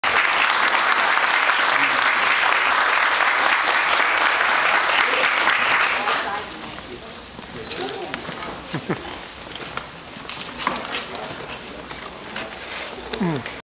Christopher Plummer CinemaParadiso FLiFF 012
In tribute to "Wind over the Everglades," Christopher Plummer visited Cinema Paradiso in Fort Lauderdale, Florida.

Plummer receives many gifts from Mary Osceola and the Seminole Tribe, far more worthy than the Oscar.